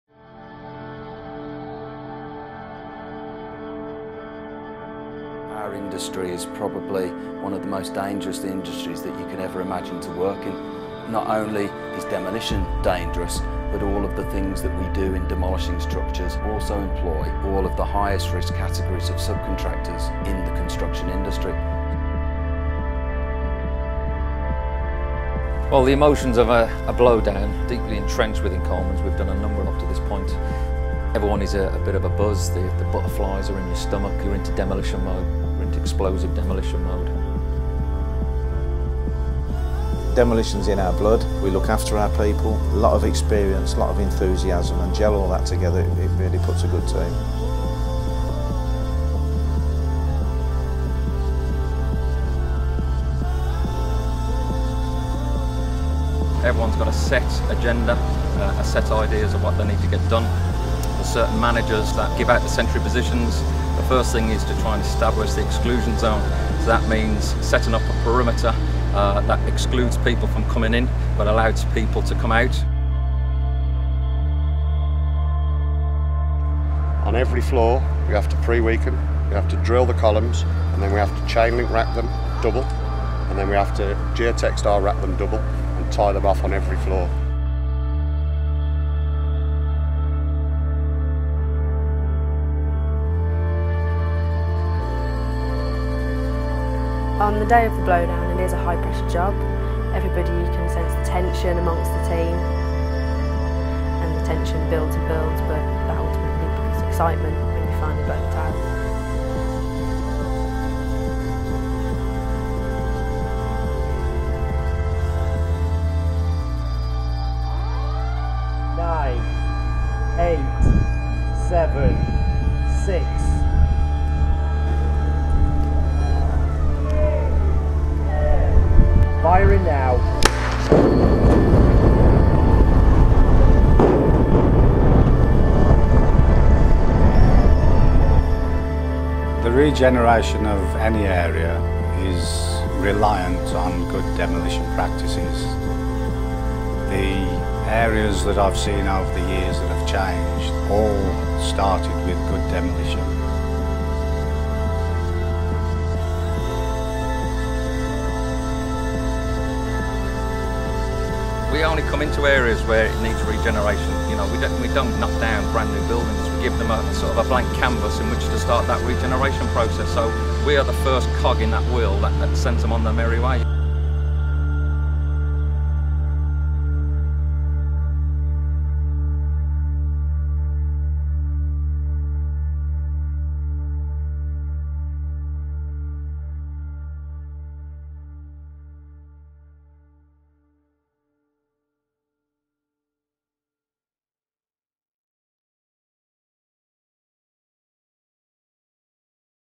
Coleman & Company: Explosive Demolition; Sandiefield Road blowdown experience
Coleman & Company's detonation of two twenty-four storey tower blocks in Sandiefield Road, Gorbals, Glasgow, UK. Experience the the rising tension and excitement, as the expert team prepares to bring down the blocks, and the awesome spectacle of the towers crashing down to the ground.

The team at Coleman & Company would like to thank everyone who worked tirelessly to make this project such a great success. Without the knowledge, enthusiasm and experience of every single member of the team it simply would not have been possible. Thank you.